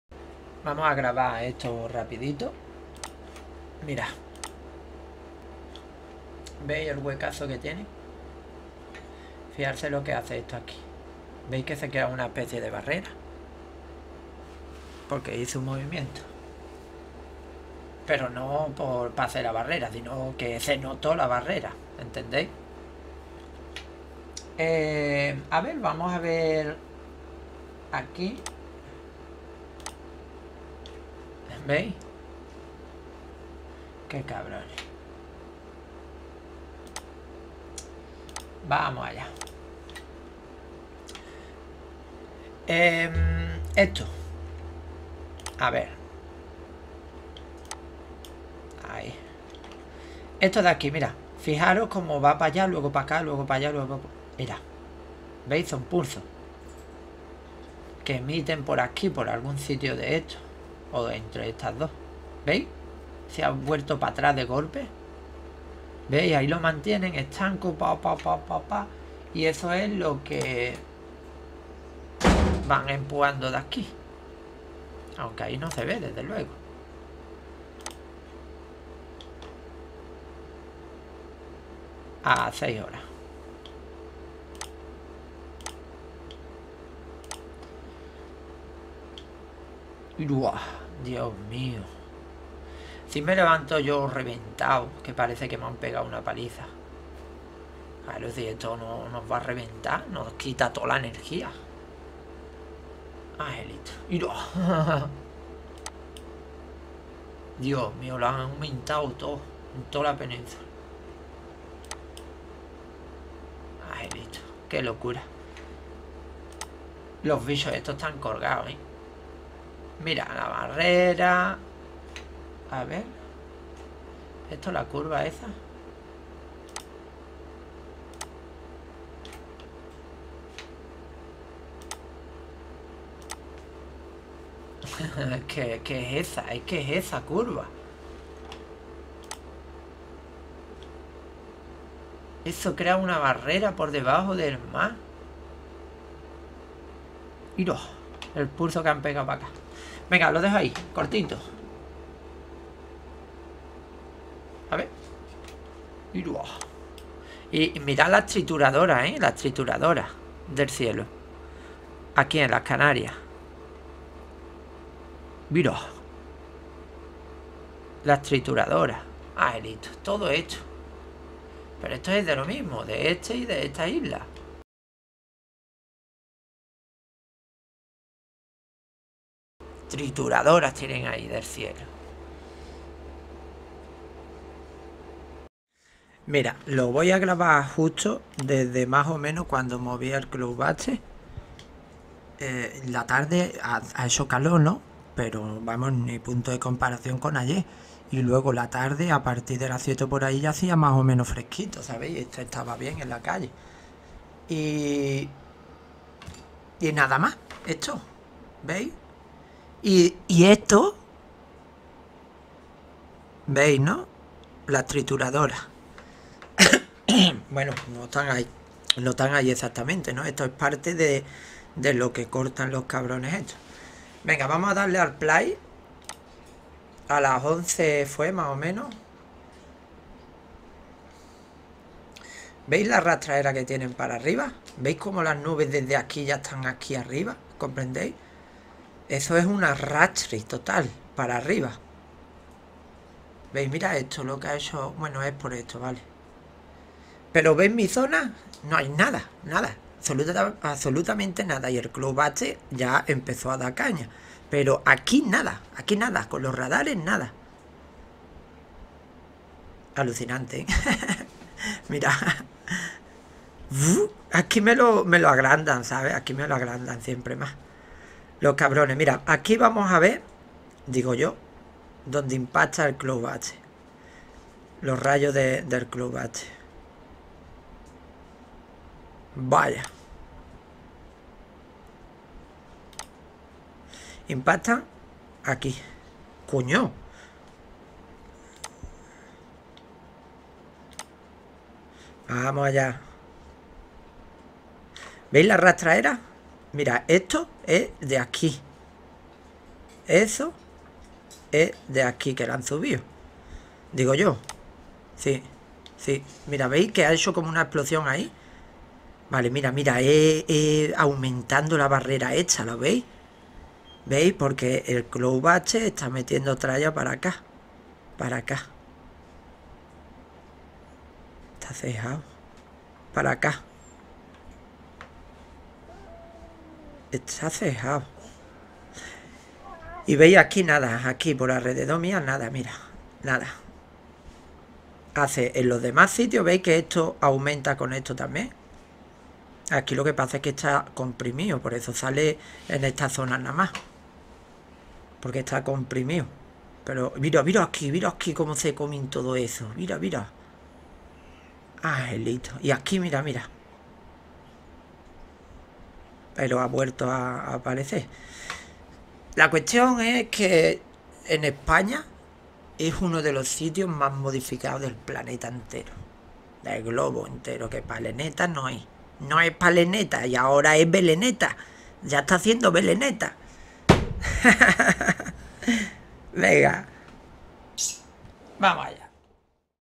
"2020-06-30" Los 5 Gatitos
Ahí están, dando traya, expulsando la humedad de la tierra, este video es cortito, es de hoy, un video simple, porque tampoco hay mucho que ver jajajaja.